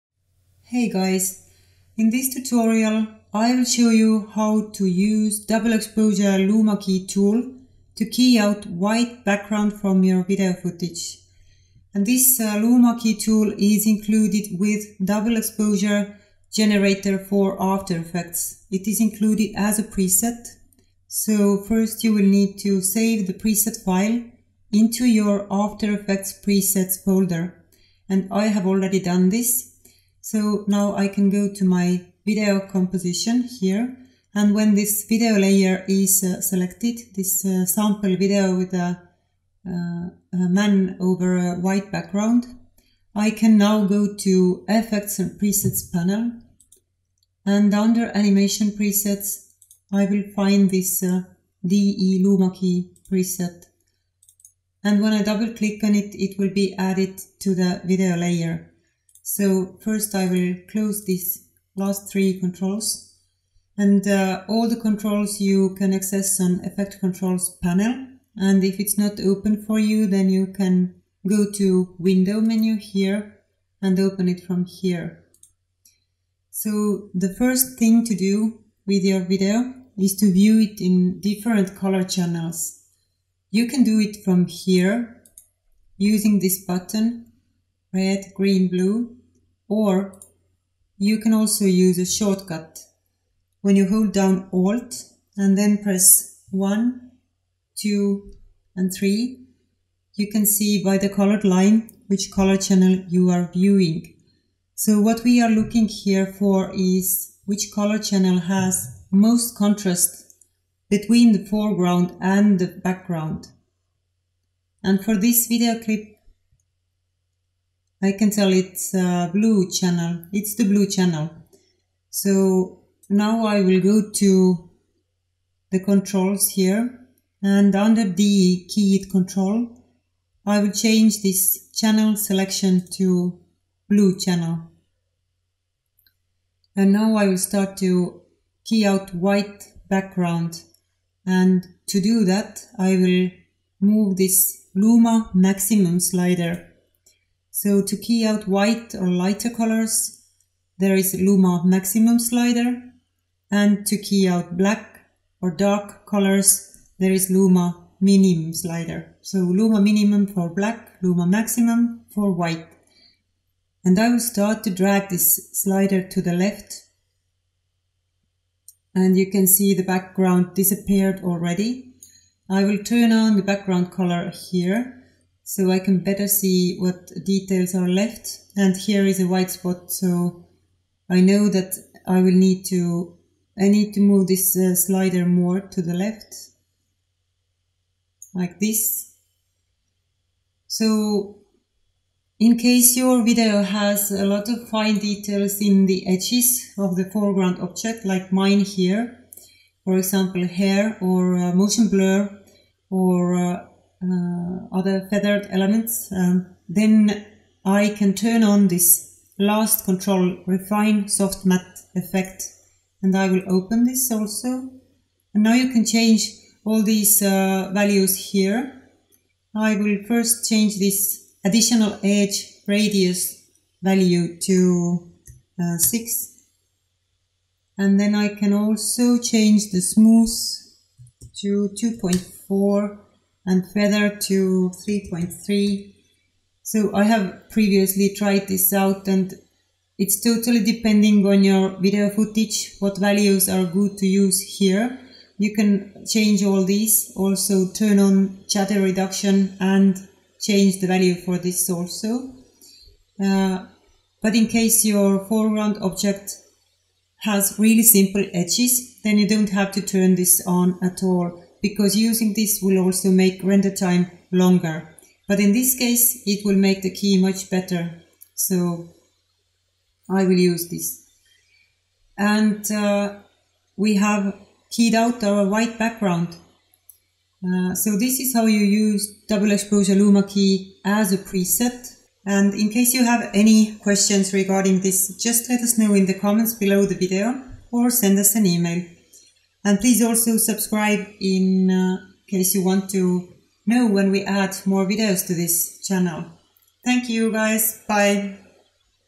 Tutorial 1.3 How to Remove White Background with DE Luma Key Tool
How to remove white background from video with DE Luma Key Tool preset for After Effects.
DE Luma Key Tool is included with Double Exposure Generator for After Effects.